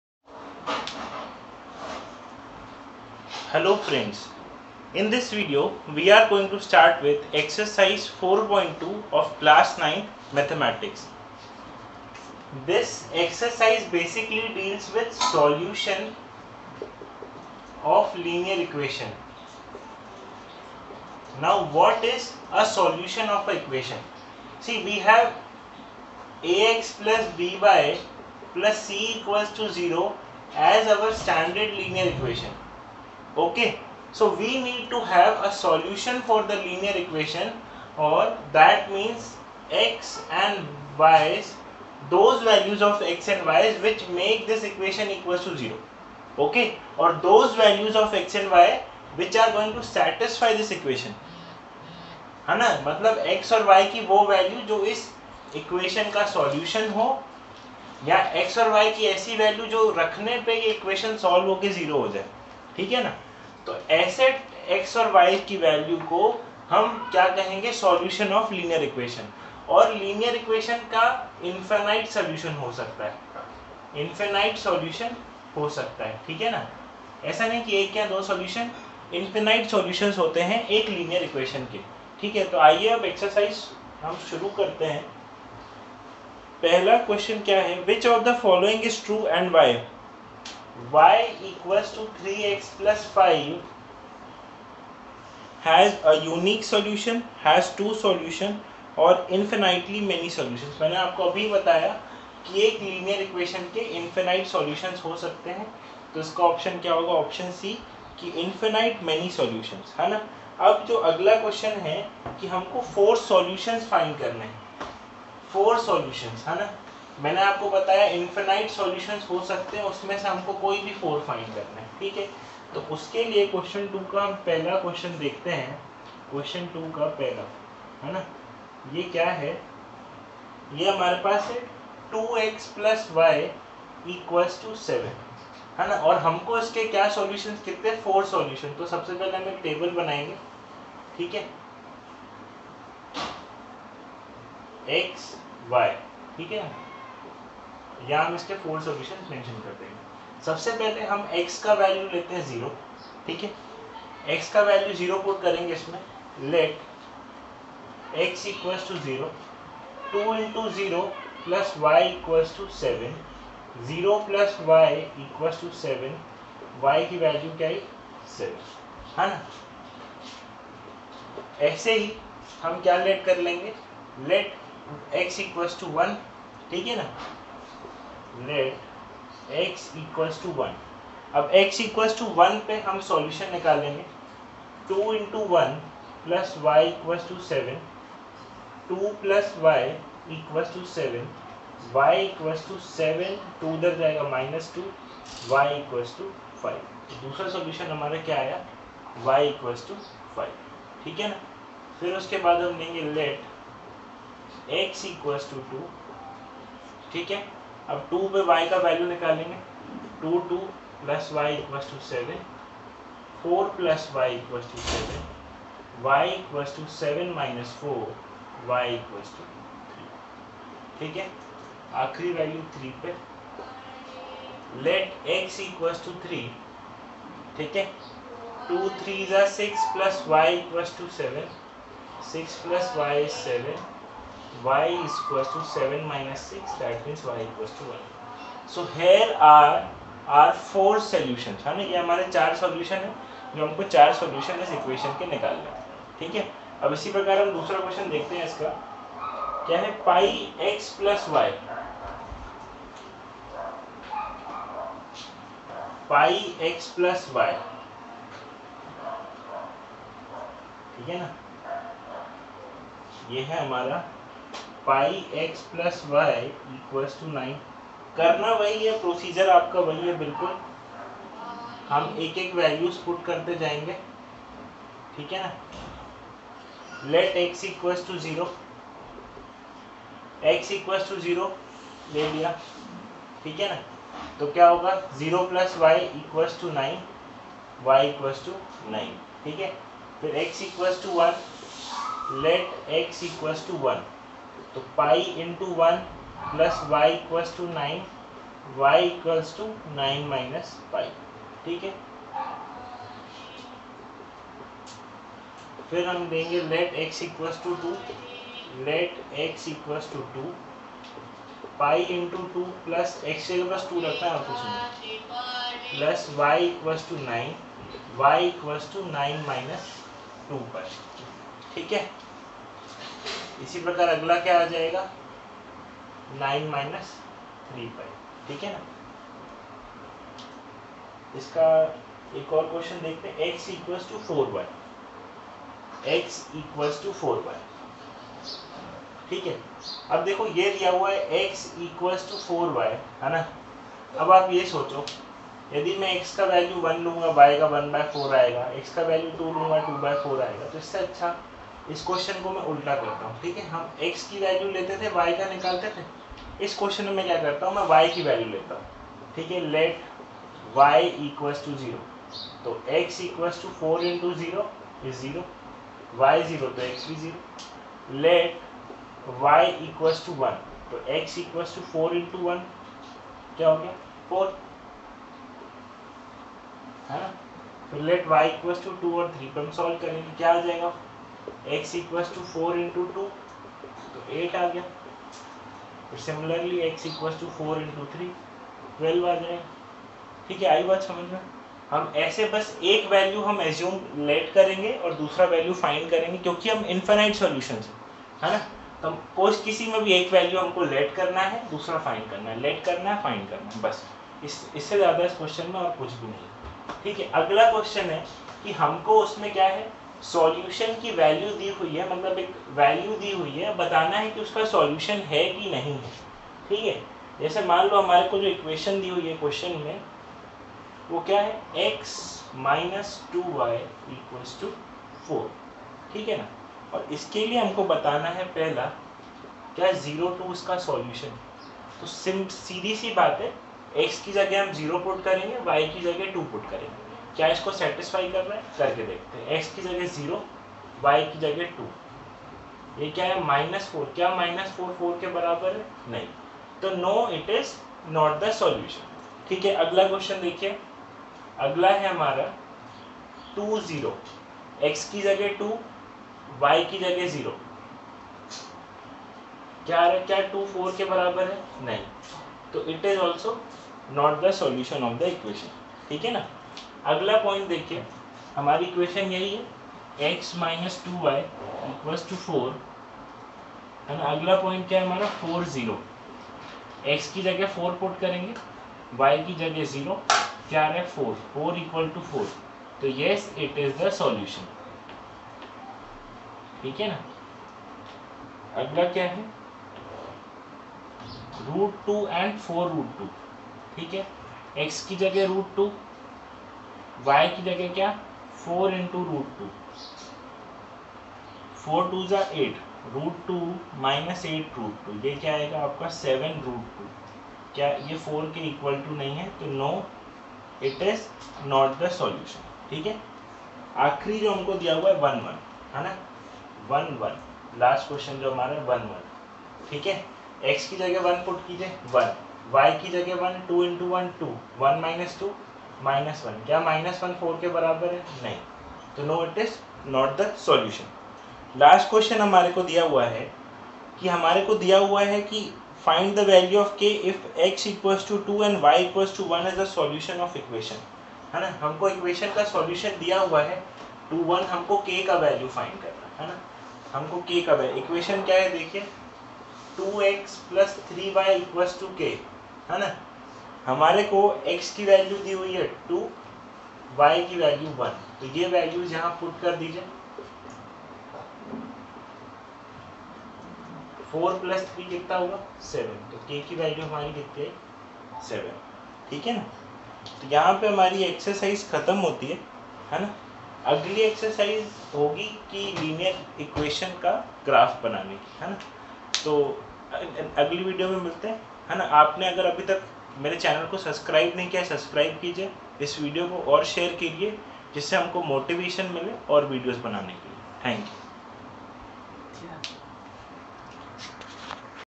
EXERCISE 4.2 NCERT CLASS 9 SOLUTION OF LINEAR EQUATIONS.
Deals with detailed solutions of each question of exercise 4.2 of NCERT CLASS 9 mathematics
LINEAR EQUATIONS IN TWO VARIABLE